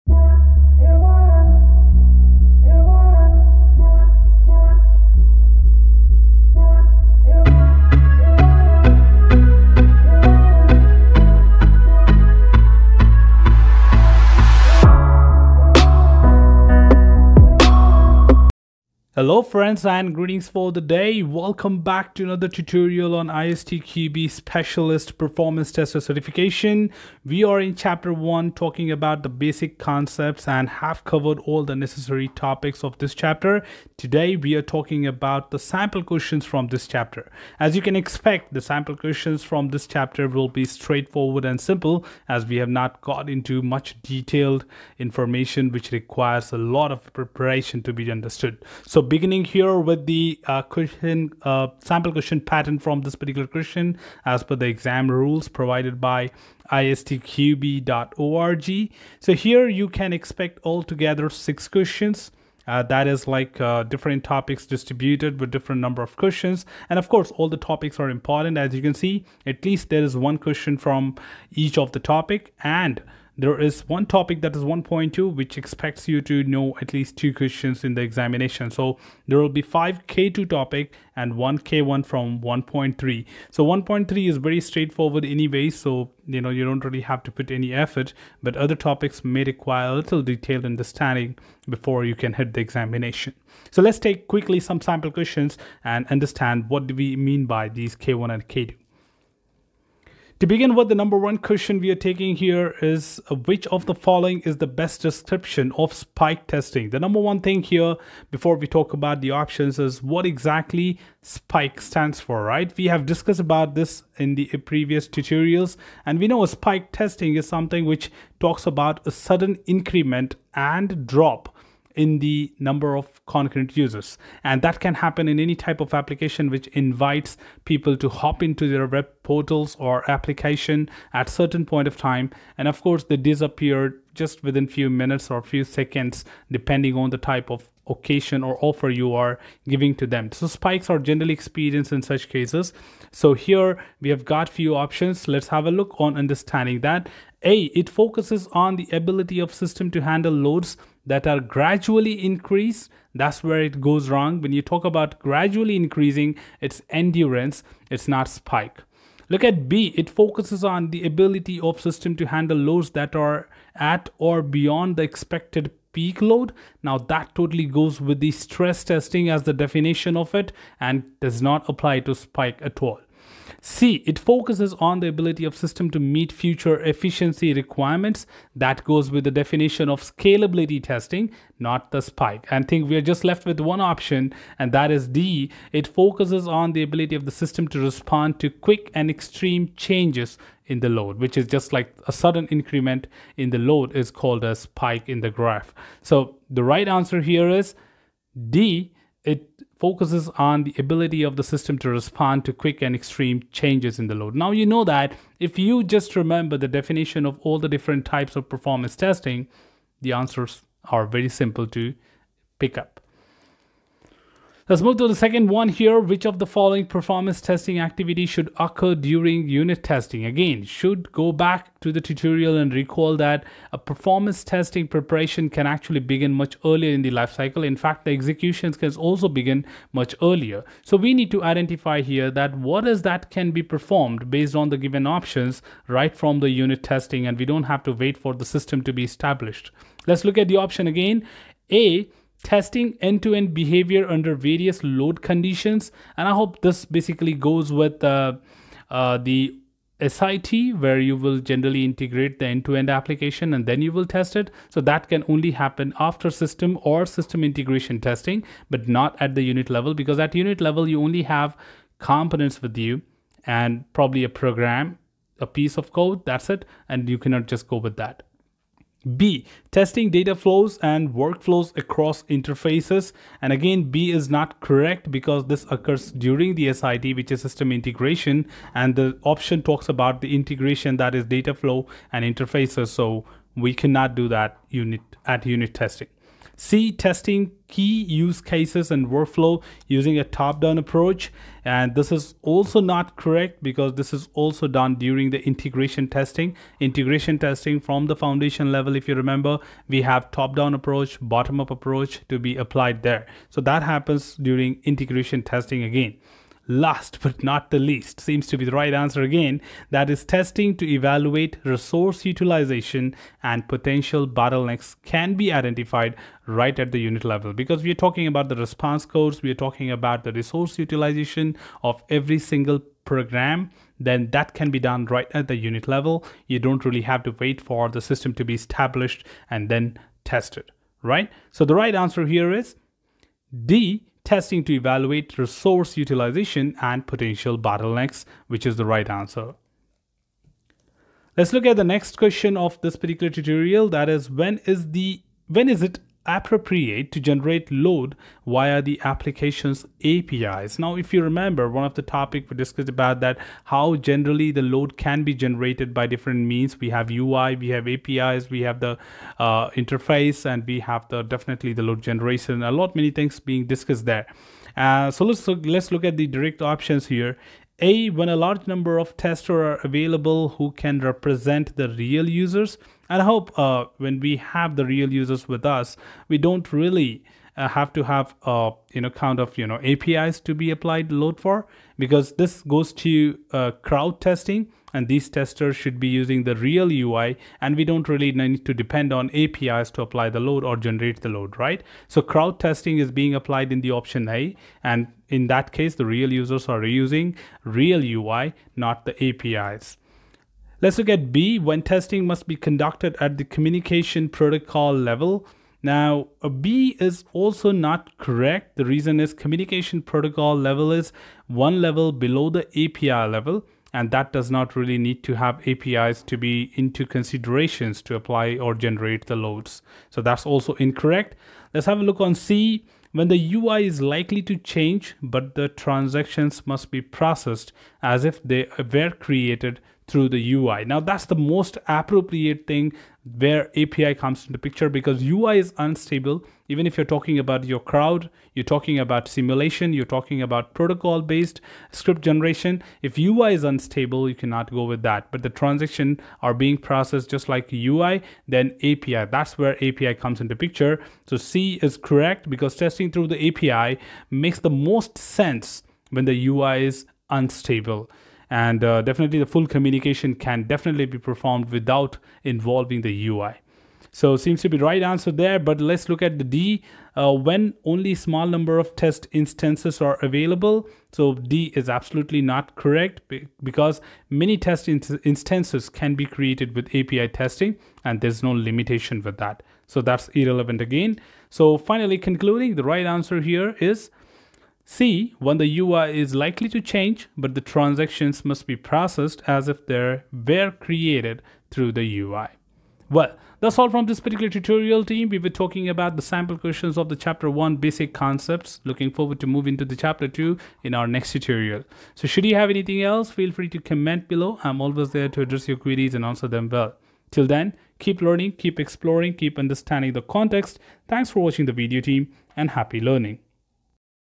ISTQB Performance Tester | Sample Questions on Chapter 1 | ISTQB Tutorials
Hello Friends,

This tutorial will drive individuals about the Sample Questions of Chapter 1 - Basic Concepts of ISTQB Specialist Performance Tester Certification of ISTQB Examination.

This series of tutorials will help individuals to prepare for the ISTQB Specialist Performance Tester exam. This series consists of ISTQB Syllabus discussion, understanding, tips, tricks, and time management about the exam by Neeraj Kumar Singh.

ISTQB Specialist certification,
ISTQB Performance Tester Tutorials,
Performance Testing Certification,
Performance Testing Sample Questions,